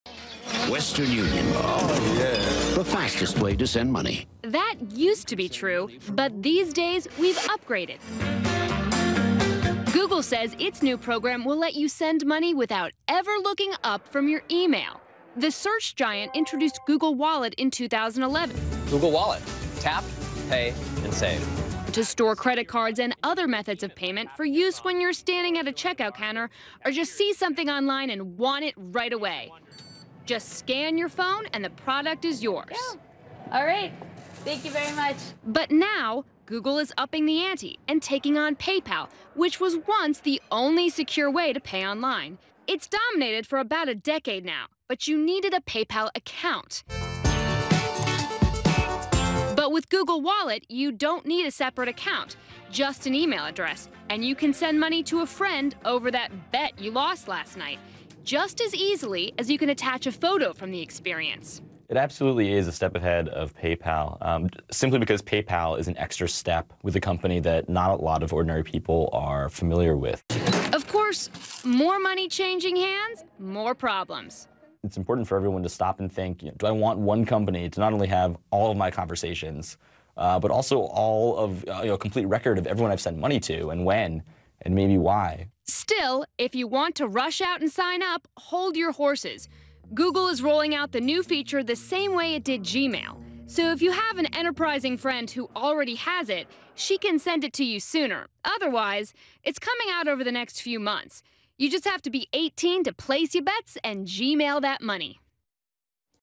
Will Google's e-commerce 'e-rase' PayPal?
Google unveiled a new update to some old technology, making it faster, easier to exchange money using a virtual wallet.